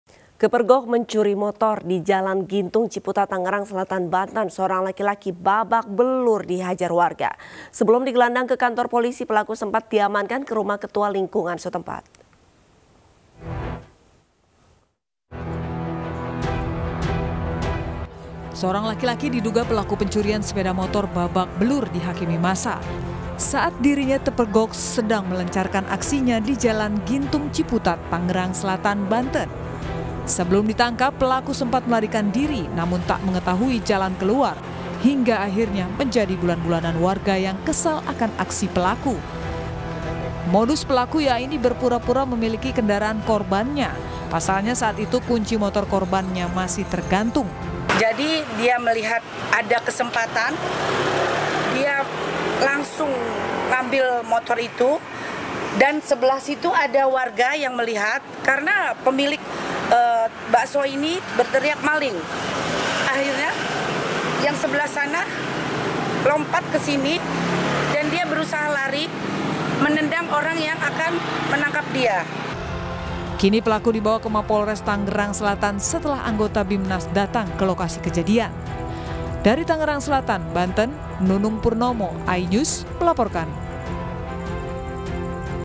Maling Motor Babak Belur Diamuk Massa di Tangerang Selatan, Banten
Seorang pria babak belur dipukuli warga saat tertangkap basah mencuri motor. Terduga pelaku pencurian motor itu ditangkap warga saat memulai aksinya di  rumah warga di Tangerang Selatan. BZK